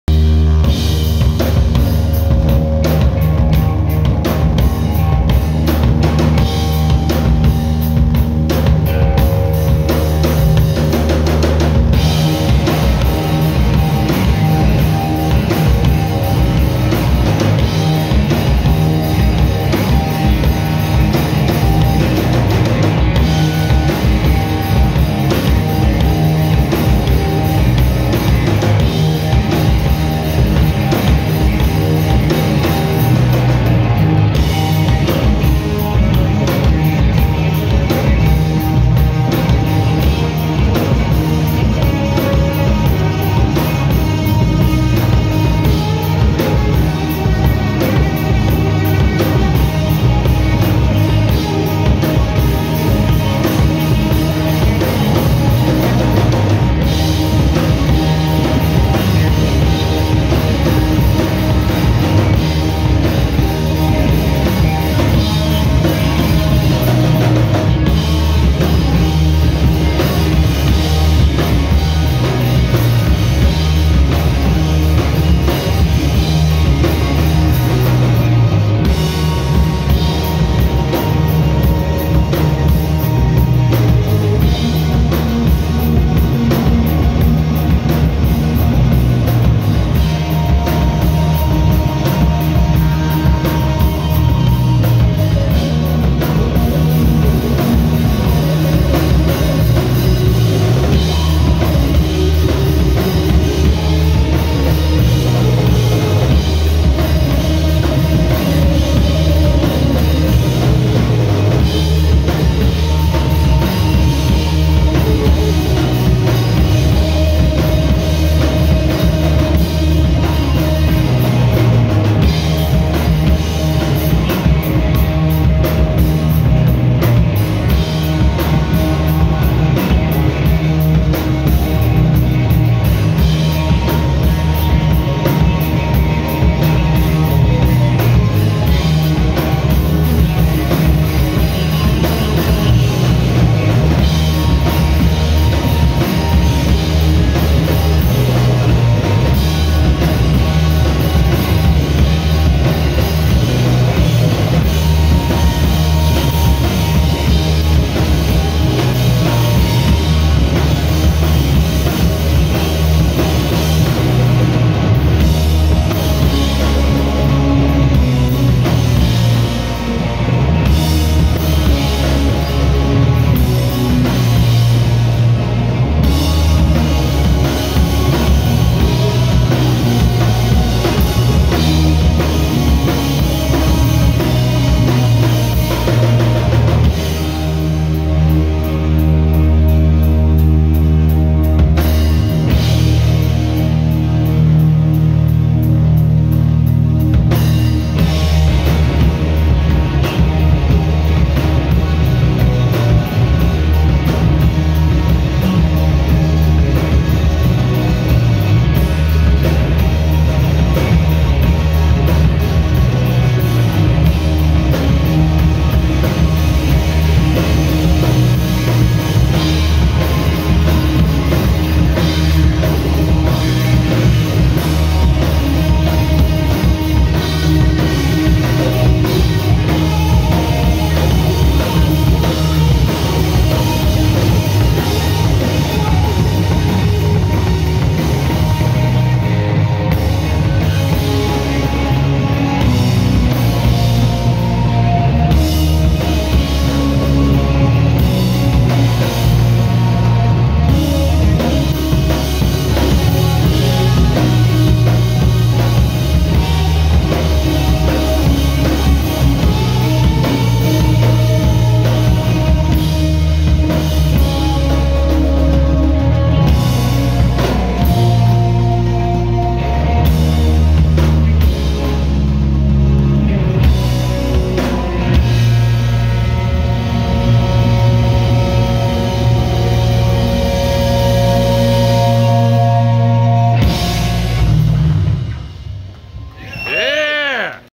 Severed Satellites at DC Brau on 7/10/22
Severed Satellites performing live at DC Brau brewery on Sunday 10 July 2022. This show was part of their Dock Days of Summer series where the bands played from the loading docks at the brewery. I actually booked this show myself.